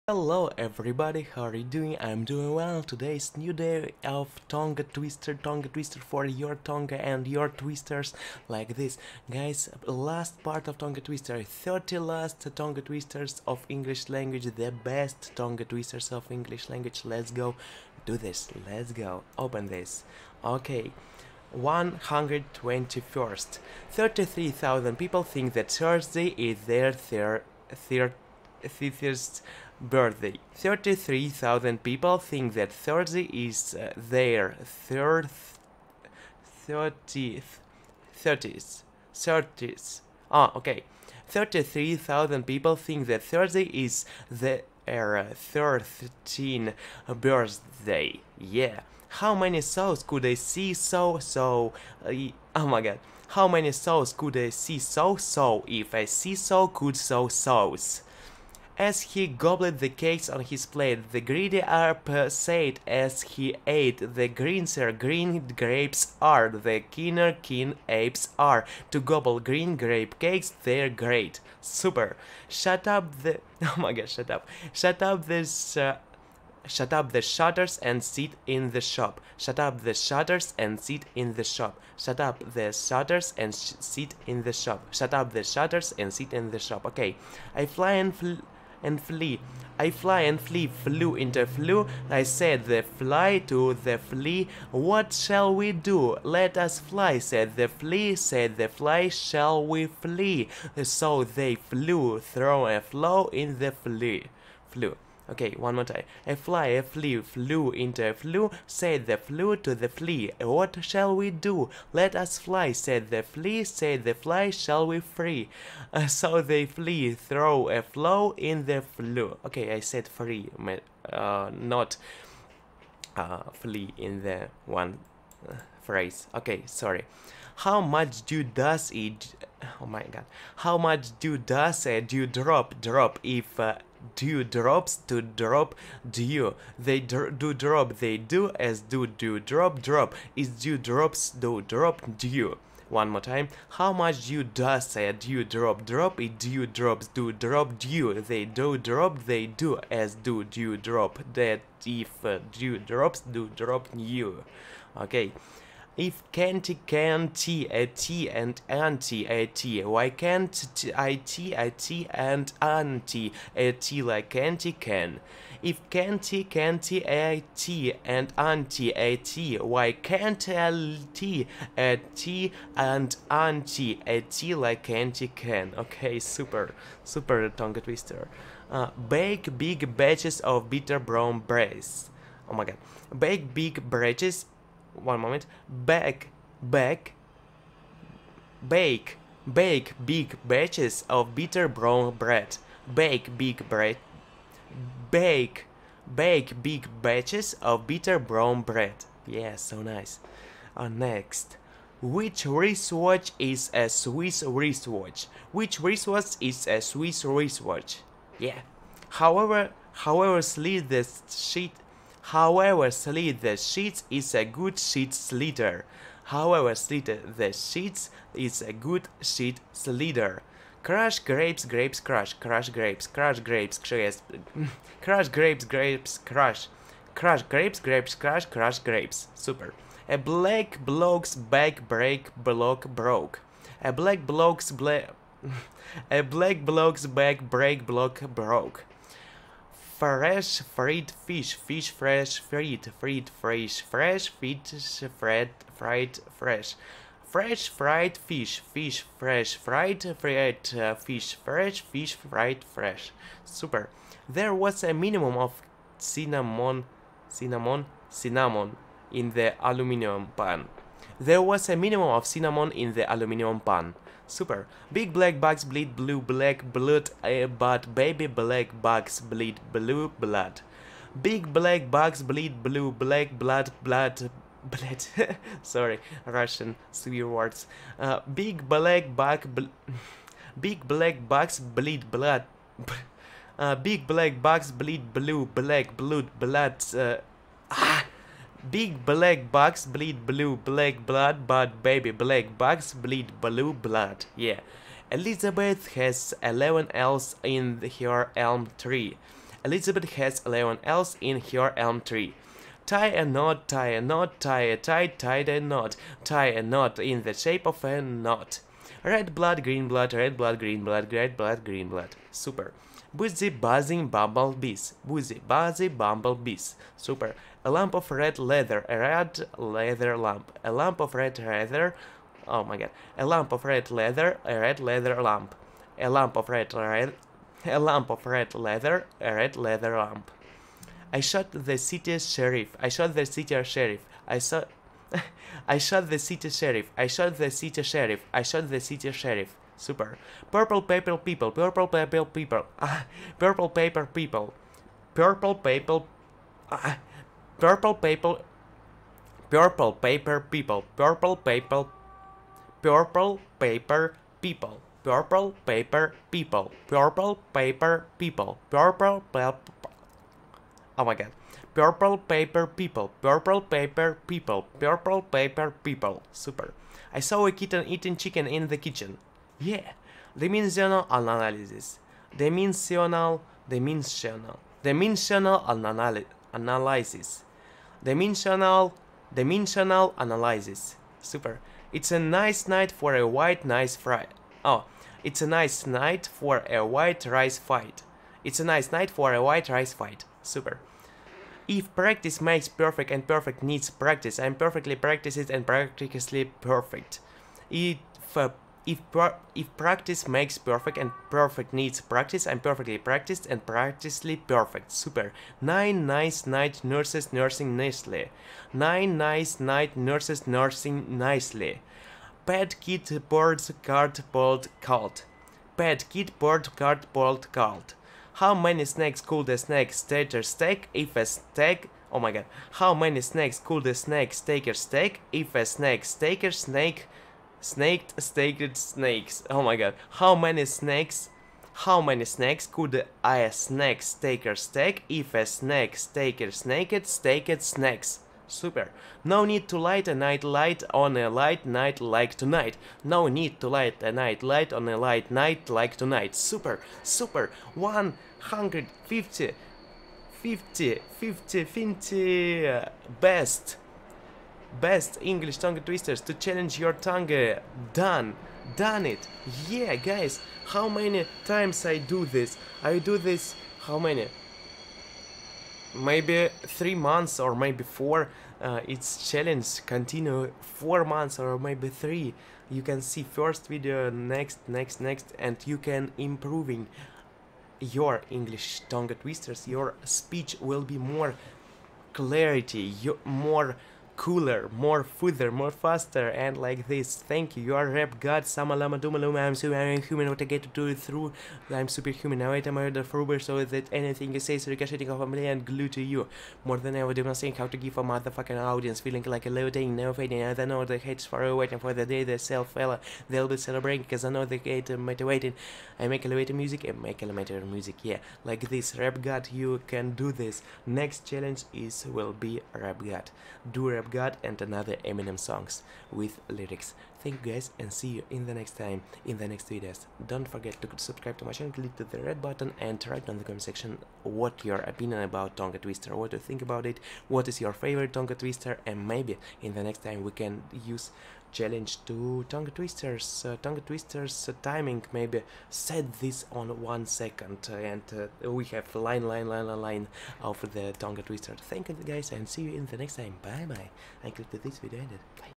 BEST 150 ENGLISH TONGUE TWISTERS | RUSSIAN MAN TRY TO SPEAK THIS | PART 9. LAST!
Thanks :)